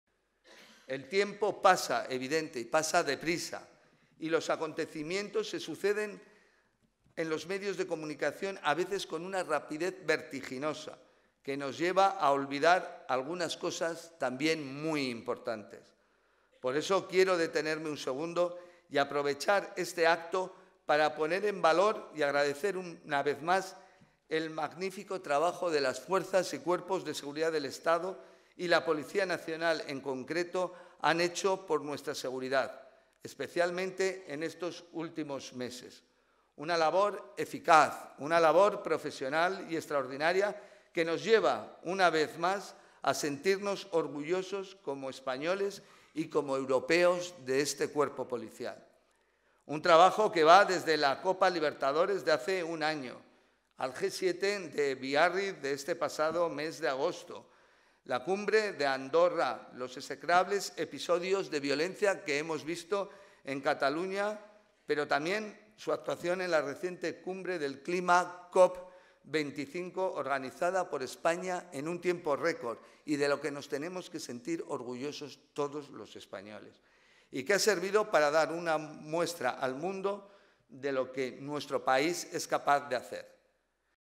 Marlaska agradece la labor de la Policía en estos últimos meses
El ministro del Interior en funciones, Fernando Grande-Marlaska, ha presidido el acto de jura de la XXIX Promoción de ascenso a la categoría de comisario de la Policía Nacional donde ha dado las gracias a la policía por lo que han hecho especialmente durante estos últimos meses.